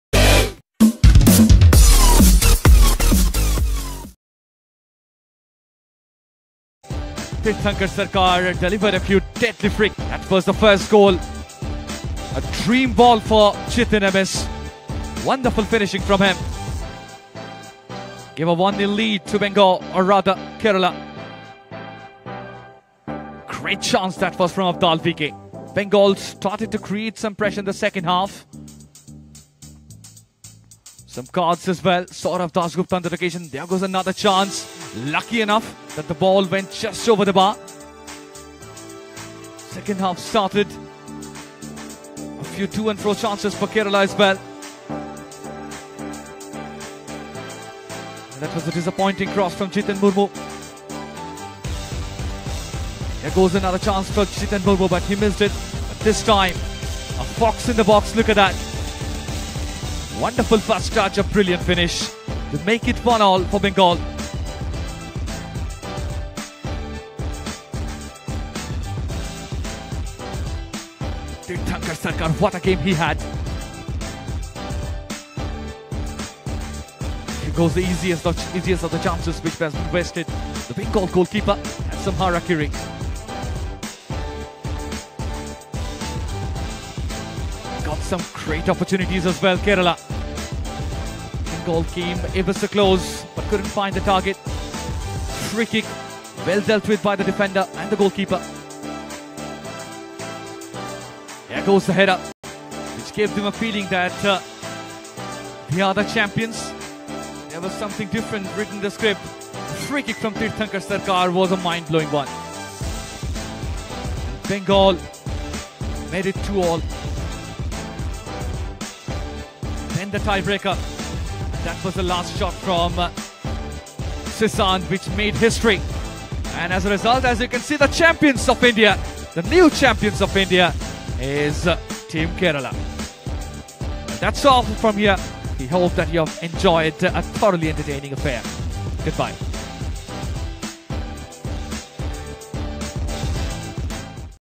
Kerala vs Bengal Final Highlights | സന്തോഷ് ട്രോഫിയിൽ ആറാം തവണയും കേരളം മുത്തമിട്ടു... 💐💐
Kerala_team_congrats_welldone,❤❤❤